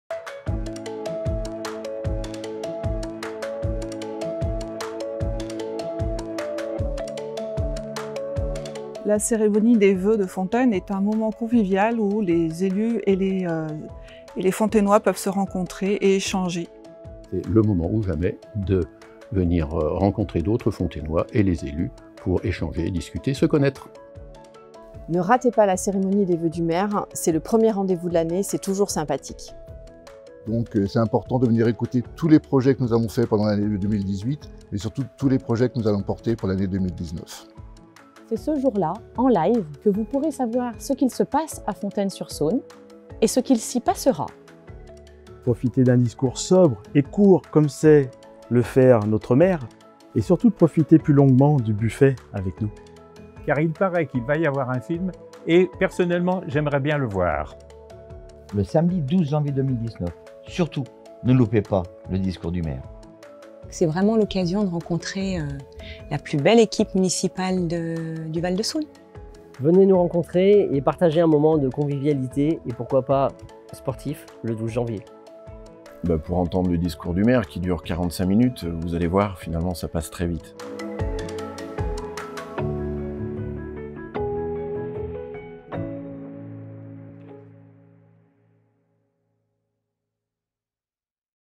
Pourquoi participer à la cérémonie des vœux du Maire 2019 ?
Les élus vous proposent différentes raisons de venir partager ensemble ce moment, premier temps fort de la nouvelle année. Venez nombreux samedi 12 janvier à 11h à la salle des fêtes (34 rue Pierre Carbon).

Vidéo réalisée par La Brèche Studio